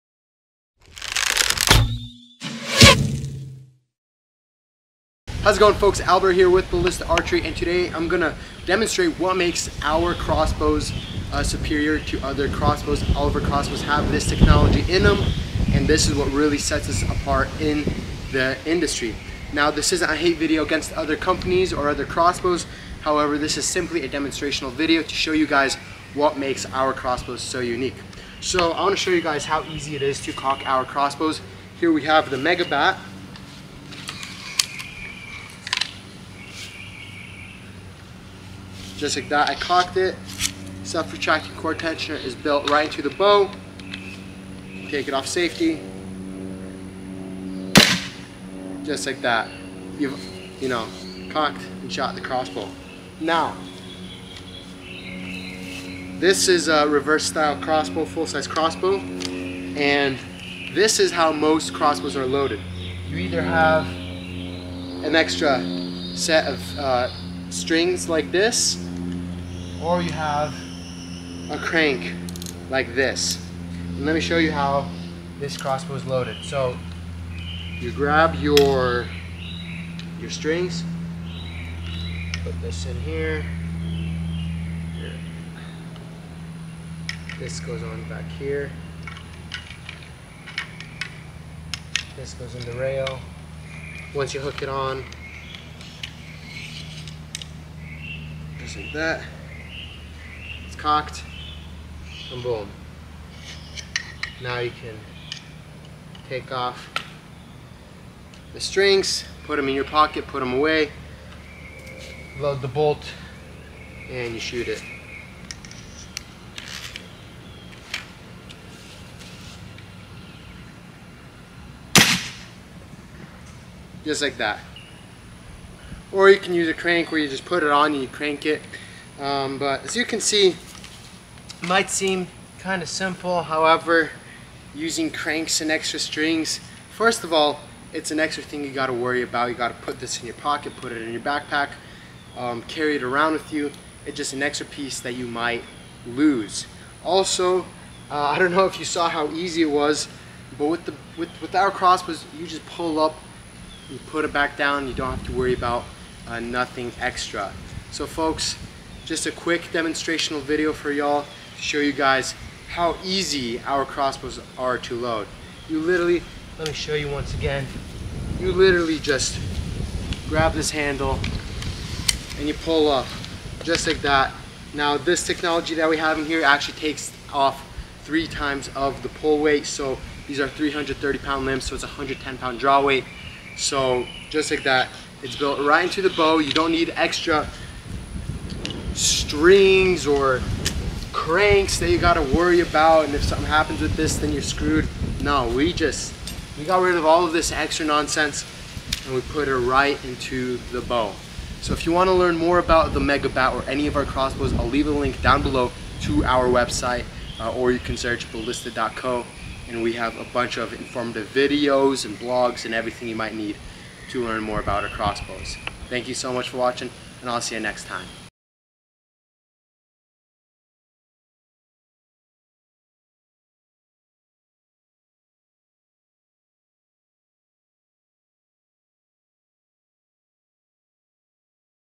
What makes Ballista crossbows superior to others
Here's a quick demonstrational video on what makes Ballista crossbows superior to others. The Built-in self-retracting cord tensioner is built right into the bow reducing the need for extra strings or crank.
This means you can spend less time cocking the bow and worrying about misplacing those instruments and more time doing what you love!

Have questions?